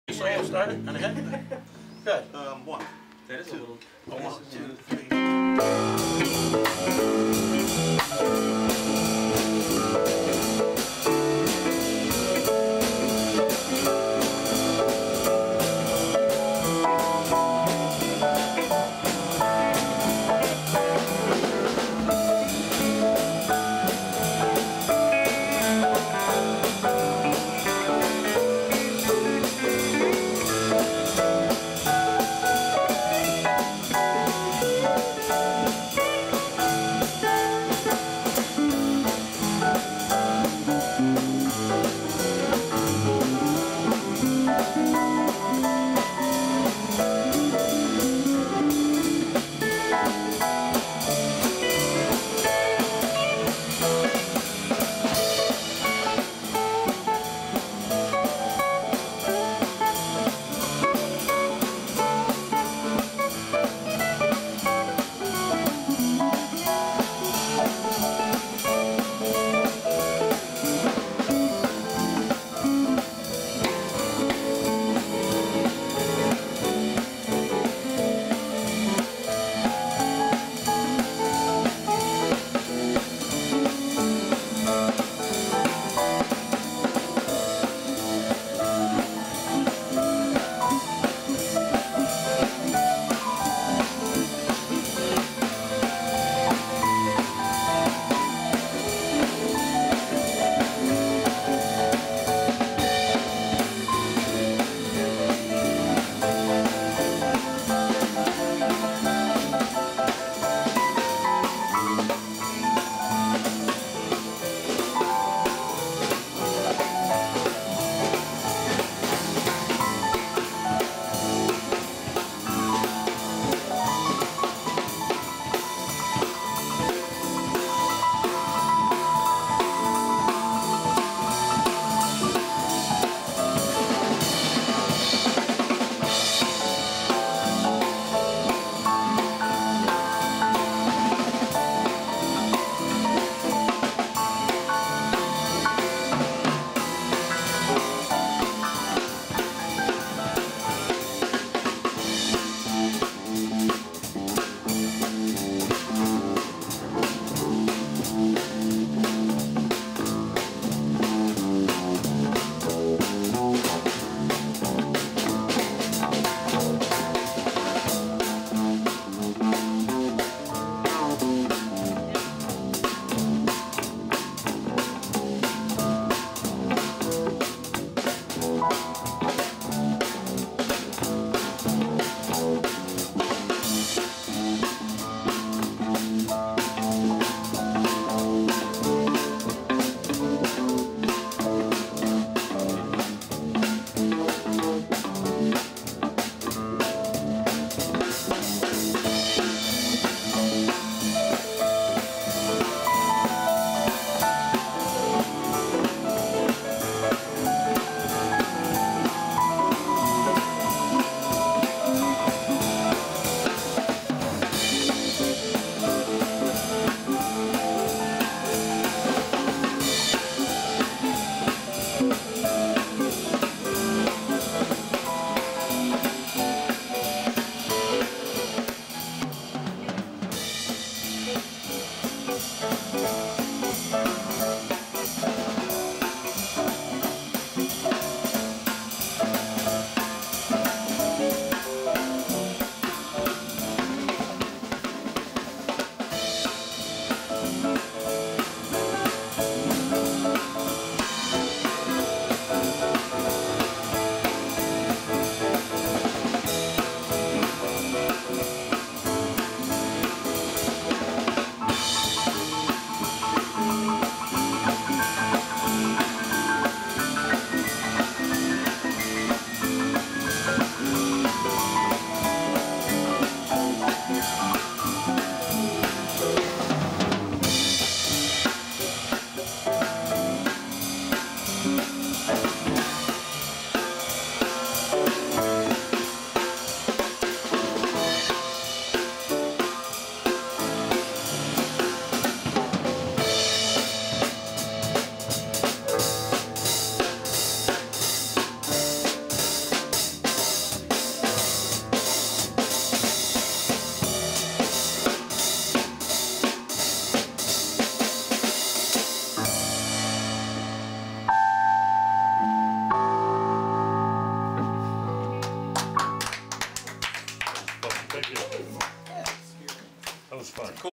The Music Score's 10-29-2010 Open Mic, John Lomartra and his trio perform with Geoff Bell on bass
John Lomartra and his trio perform with Geoff Bell on bass
More  video from our monthly Open Mic on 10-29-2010. John Lomartra on piano, Phil Devit on guitar, Matt Vernalie on drums and Geoff Bell on bass.
Our Monthly Open Mic is FREE and on the last Friday of every month. Friend us on FaceBook!